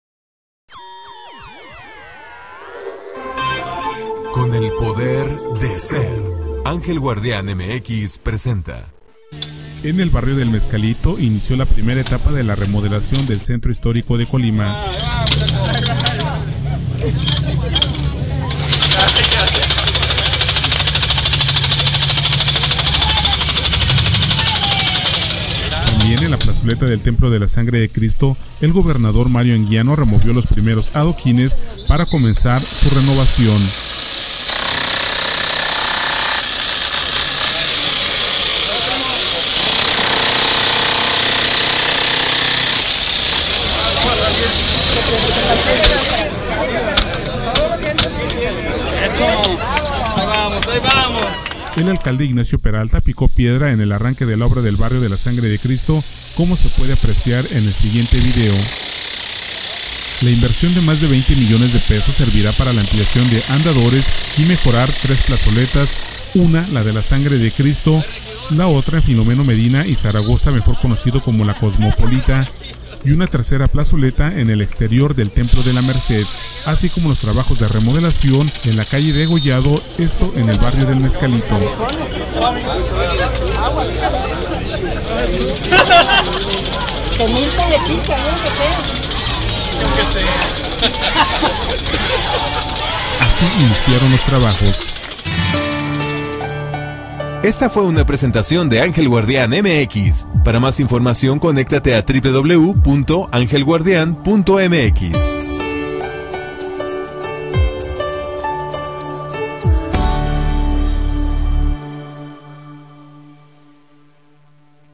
Barrios tradicionales de Colima en remodelación
El propósito de los trabajos es ofrecer una mejor cara de la capital a los turistas y que los colimenses disfruten de un centro histórico más bonito, cómodo y seguro. El gobernador y el alcalde iniciaron la talacha.